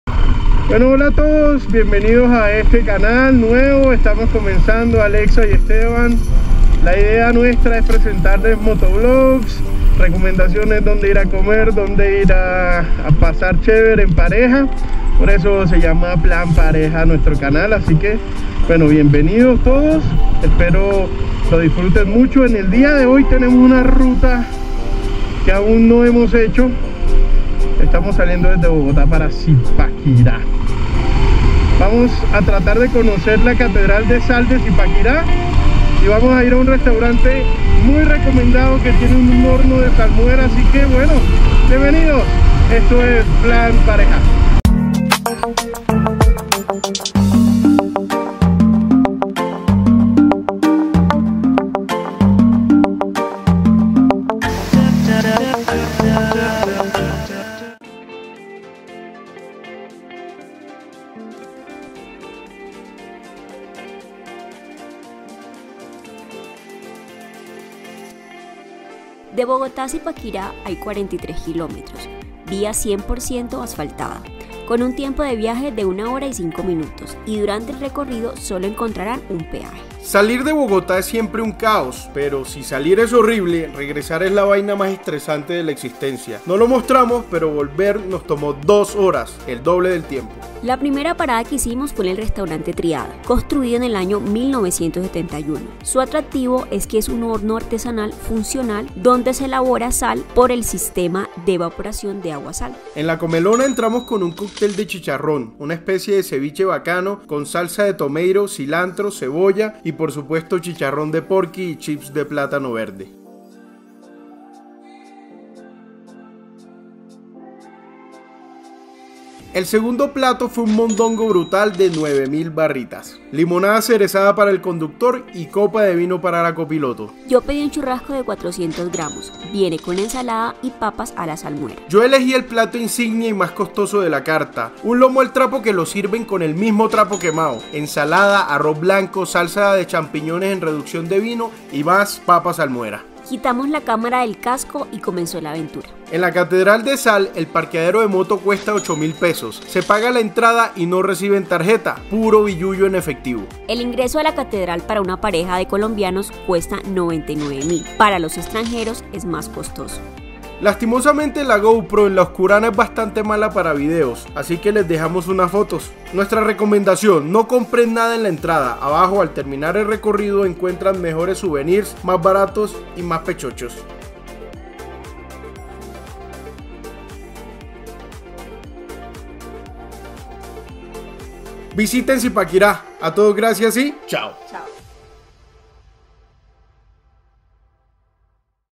PLAN PARA PAREJAS | Zipaquirá - Cundinamarca | Restaurante TRÍADA Y CATEDRAL DE SAL.
Hola, somos @alexamarquez2408 y @uparela.esteban, esta es nuestra recomendación para ir a almorzar a Zipaquirá y conocer la Catedral de Sal.

DURACIÓN: 1/2 DÍA.
PRESUPUESTO: $99.000 INGRESO A LA CATEDRAL DE SAL
PARQUEADERO DE MOTO: $8.000
RESTAURANTES: PIDIENDO LO MÁS COSTOSO $170.000. EN TRÍADA Hay platos de 20 mil y 30 mil pesos por lo que sería más económico.